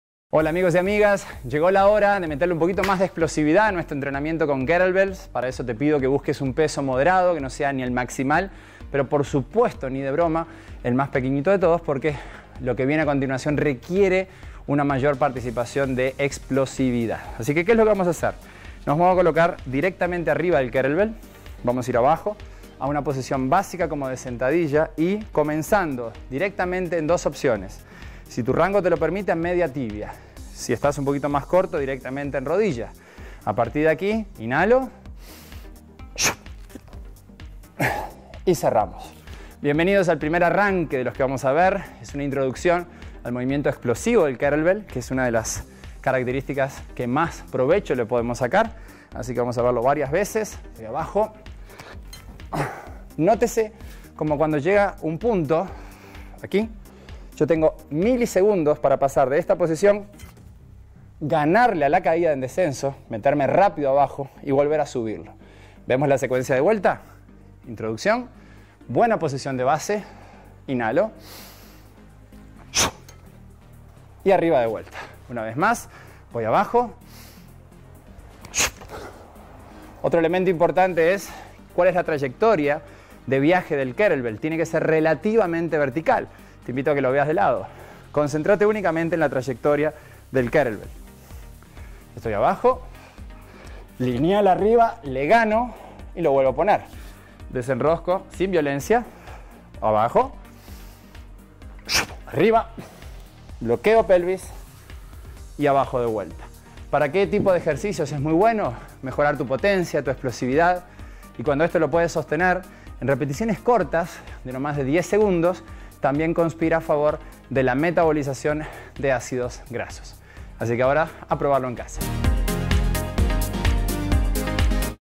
Clase 6: Arranque con Kettlebell
¡Hora de subir de nivel en nuestros ejercicios!
Hoy junto con tus Kettlebell de Telstar aprenderás las posiciones correctas de arranque y cómo sacarle mejor provecho